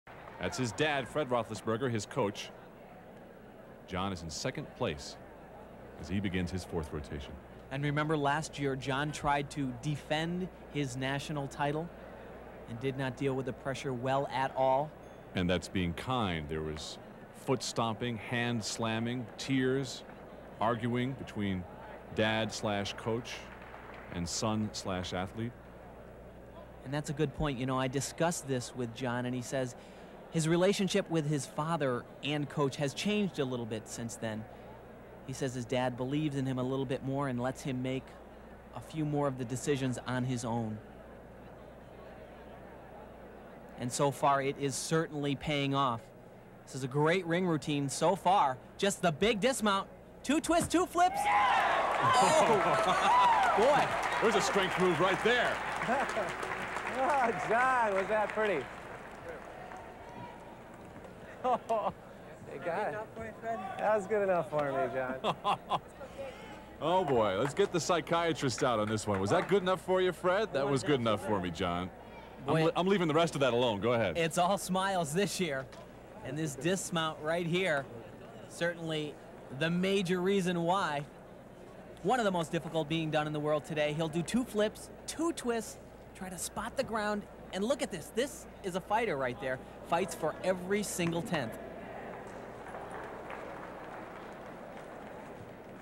John Roethlisberger - Still Rings - 1992 Phar-Mor U.S. Championships - Men
Score: 9.700
May 15, 1992 - St. John Arena - Columbus, Ohio
Gymnastics